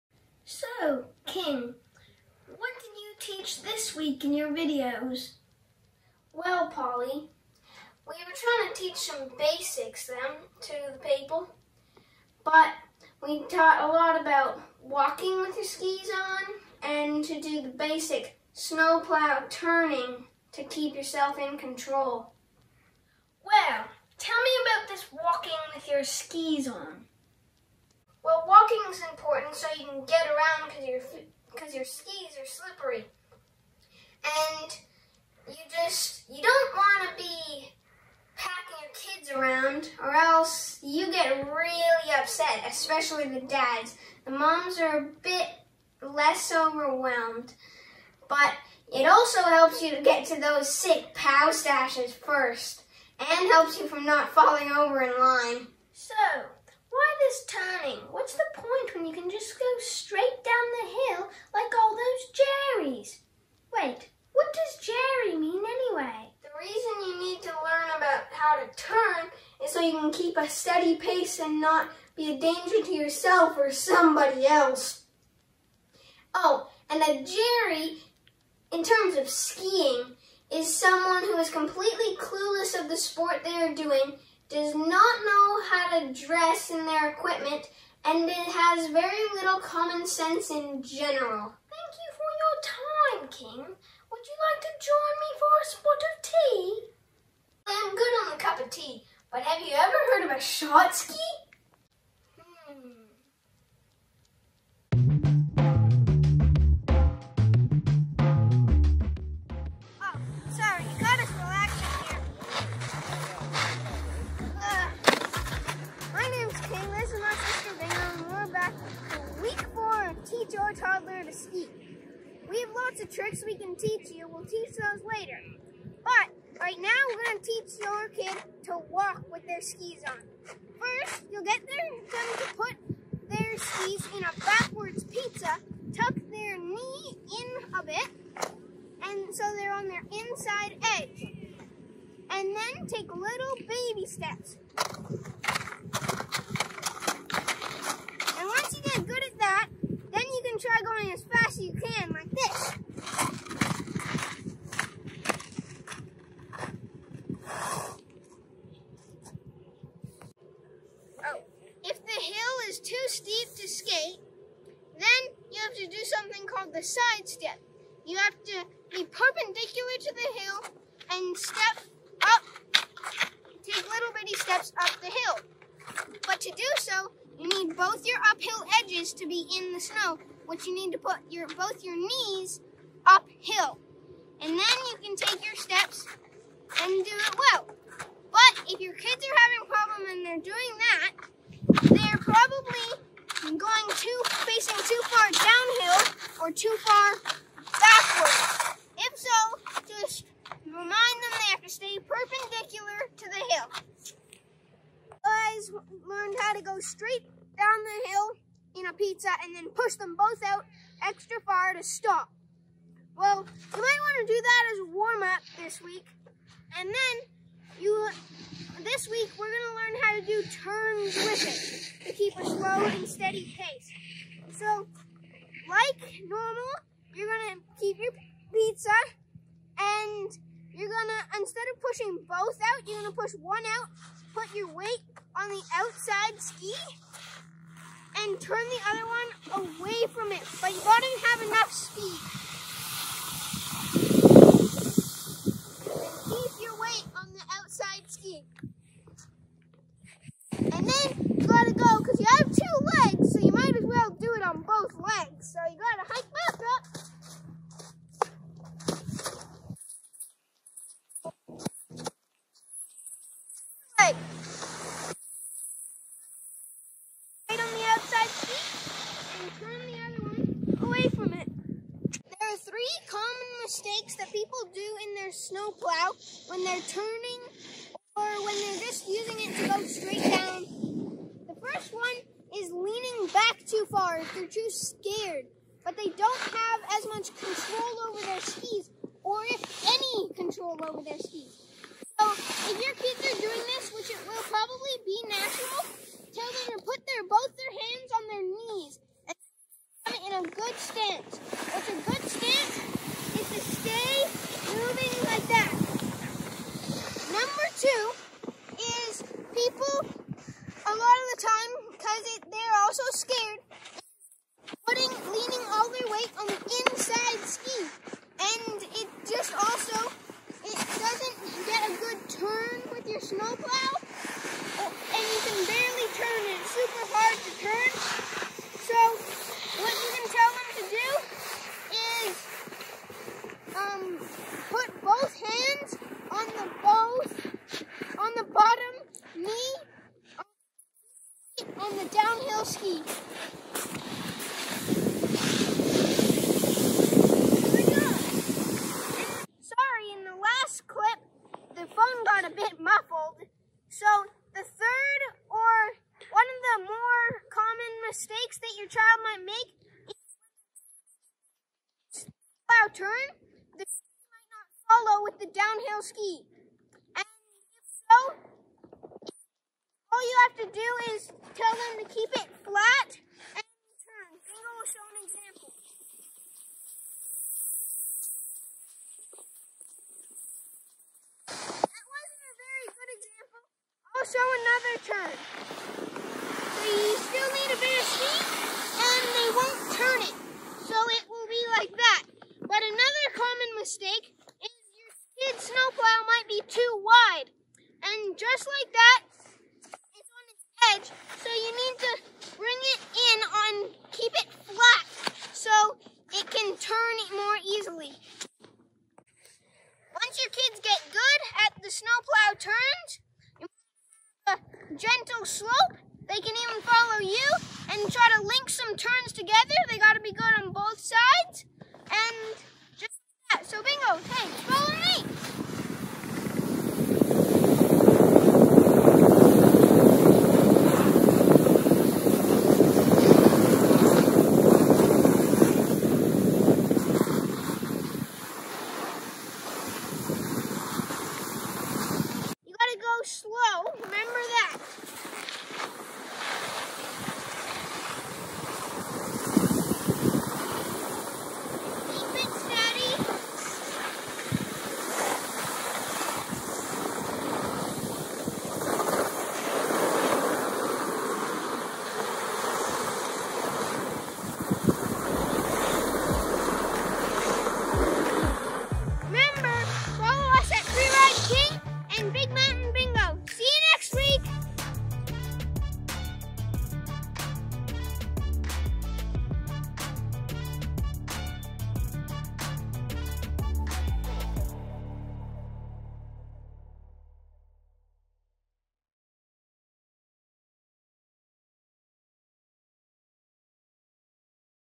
Teach your Toddler to Ski - Episode 4 - Going Down the Mountain
King and Bryn(Bingo) Eyben have tips for parents who plan on teaching their toddlers to ski this season. Using clever skits and informative tutorials, these two Pemberton rippers will have you feeling confident that you can turn your kids into little rippers too! This episode the kids hit the slopes and teach us the beginning steps of going down the mountain.